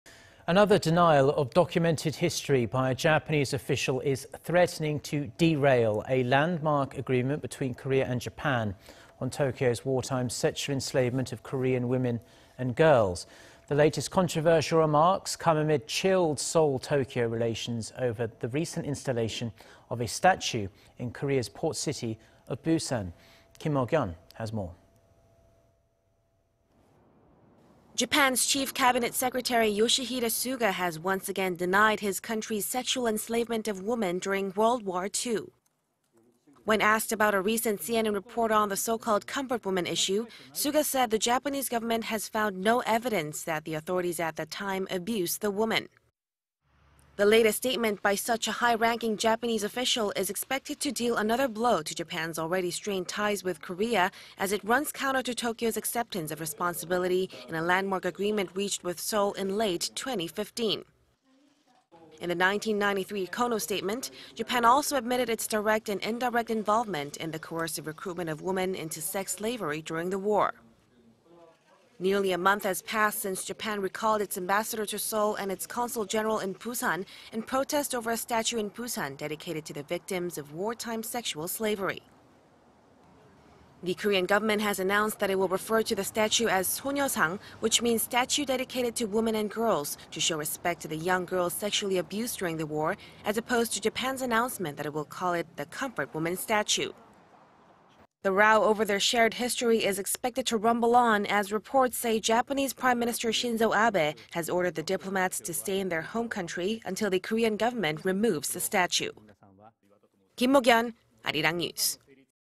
Senior Japanese official denies coercive recruitment of women into sex slavery during WWII
日 '위안부 강제동원' 또 부정 "기록 없다"
Another denial of documented history by a Japanese official is threatening to derail a landmark agreement between Korea and Japan... on Tokyo's wartime sexual enslavement of Korean women and girls.
The latest controversial remarks come amid chilled Seoul-Tokyo relations over the recent installation of a statue in Korea's port city of Busan.
Kim Mok-yeon has more.
Japan's Chief Cabinet Secretary Yoshihide Suga has once again denied his country's sexual enslavement of women during World War II.
When asked about a recent CNN report on the so-called "comfort women" issue,.. Suga said the Japanese government has found no evidence that the authorities at that time abused the women.

The latest statement by such a high-ranking Japanese official is expected to deal another blow to Japan's already strained ties with Korea, as it runs counter to Tokyo's acceptance of responsibility in a landmark agreement reached with Seoul in late 2015.
In the 1993 Kono Statement,... Japan also admitted its direct and indirect involvement in the coercive recruitment of women into sex slavery during the war.
Nearly a month has passed since Japan recalled its ambassador to Seoul and its Consul General in Busan in protest over a statue in Busan dedicated to the victims of wartime sexual slavery.
The Korean government has announced that it will refer to the statue as "Sonyeosang" which means "statue dedicated to women and girls" to show respect to the young girls sexually abused during the war, as opposed to Japan's announcement that it will call it the "comfort women statue".
The row over their shared history is expected to rumble on as reports say Japanese Prime Minister Shinzo Abe has ordered the diplomats to stay in their home country until the Korean government removes the statue.